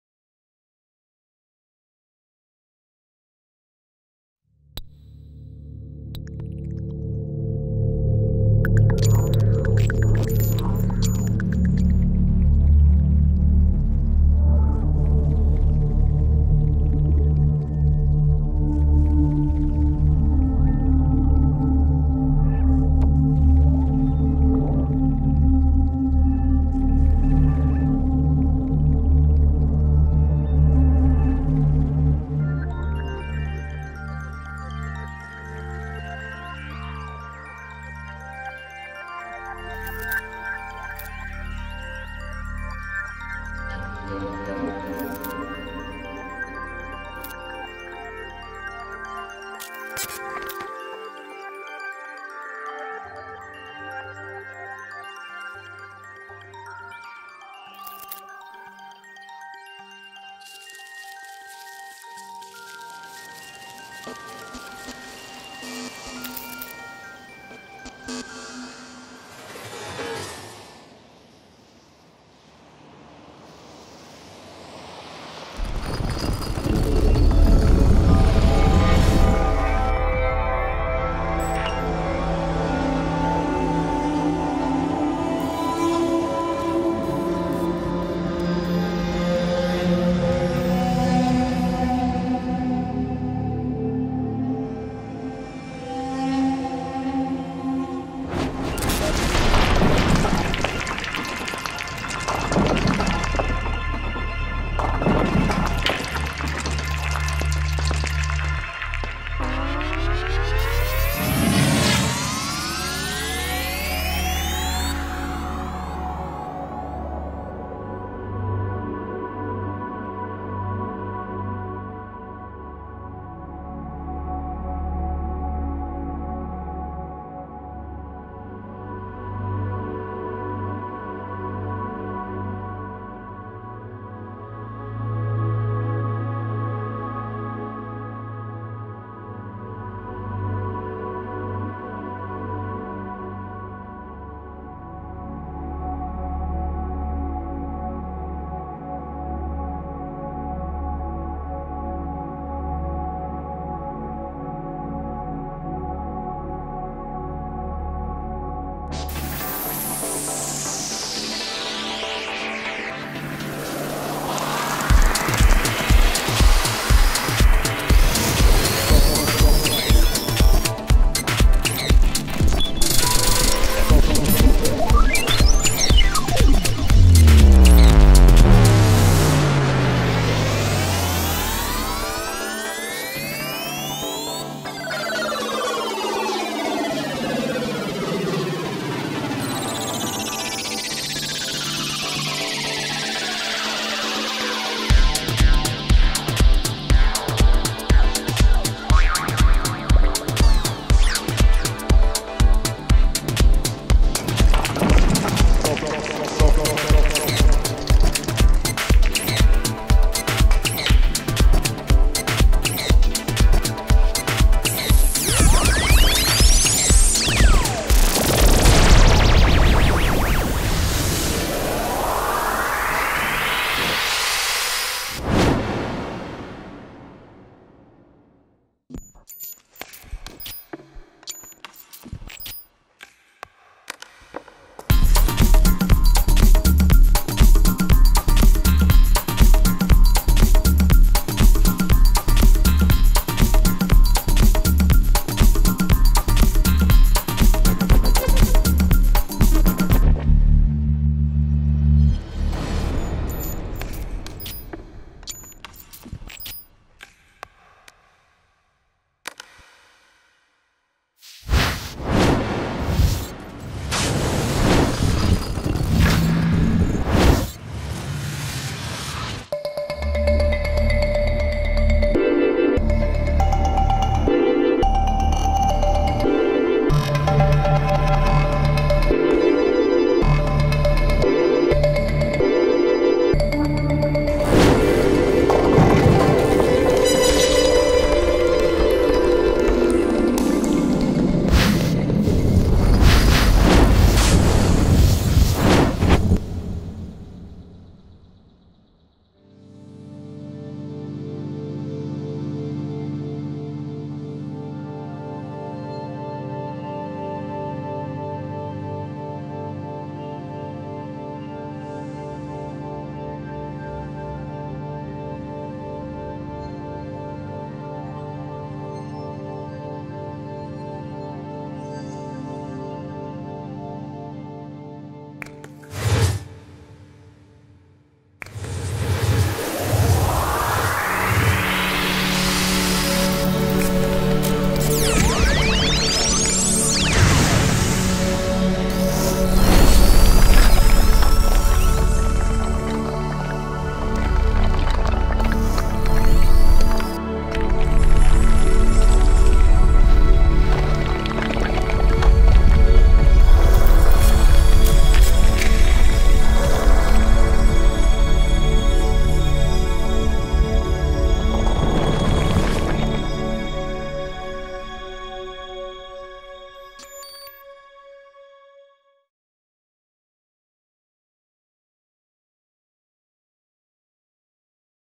Visionquest / TROYKA MULTISPACE Building projection by Radugadesign
Moscow
November, 2011
Visionquest / TROYKA MULTISPACE
Building projection by Radugadesign:
motion: Anton Novosad, Yuriy Izmailov, Weaponer
sound: Alexandr Knyazev
art-direction: Ivan Nefedkin